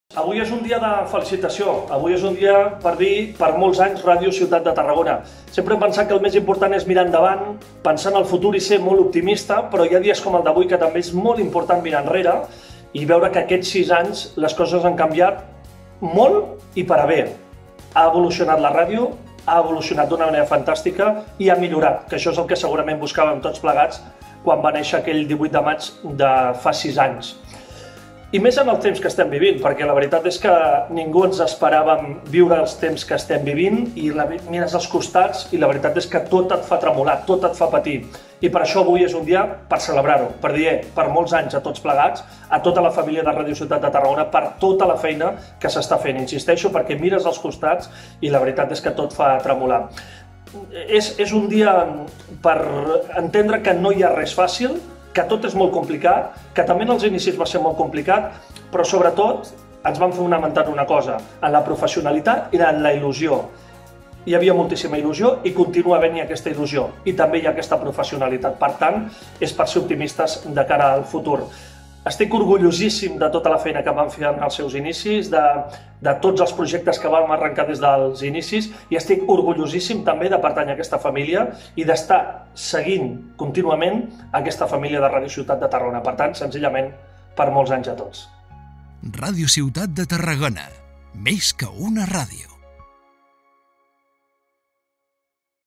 Més que mai, ho hem de celebrar
Els aniversaris sempre s’han de celebrar. Sempre. Siguin les circumstàncies que siguin, sempre hem de trobar aquell moment per mirar enrere i felicitar-nos. Algú pensarà que no serveix de massa, perquè el més important és mirar endavant. Però sense repassar allò que ja hem fet, no ets conscient de com canvien les coses, com evolucionen i, aquest és el gran objectiu, com milloren.

Aquest sisè aniversari s’ha de celebrar més que mai, perquè la situació que vivim és absolutament inesperada i de difícil resposta. No saps mai que passarà demà. Aquest sisè aniversari fa que ens sentim tots plegats més orgullosos que mai d’un projecte de comunicació, perquè mires al voltant i tot tremola. No és fàcil.

Com no va ser fàcil l’inici de l’aventura, perquè segurament pocs pensaven que compliria el sisè aniversari. No sé si fins i tot nosaltres, tot i que era un dels grans objectius. Aleshores era una aventura, ara és un projecte consolidat. Aleshores hi havia professionalitat, però sobretot molta il·lusió i aquest ha de ser el motor de tot plegat: la il·lusió ben acompanyada per la professionalitat. I sobretot saber-se adaptar. I en això crec que indiscutiblement i potser més que ningú Ràdio Ciutat de Tarragona s’intenta contínuament adaptar als nous temps.

Em sento especialment orgullós de ser una de les persones que, juntament amb el Quim i l’Albert, va iniciar el projecte. Em sento especialment orgullós d’haver estat intensament en els inicis fent els partits del Nàstic. I ara em sento especialment orgullós de veure com els companys consoliden i reforcen el projecte. Que no soni la veu, no vol dir que no hi sigui. Hi seré. Sempre. I tard o d’hora, hi tornarem.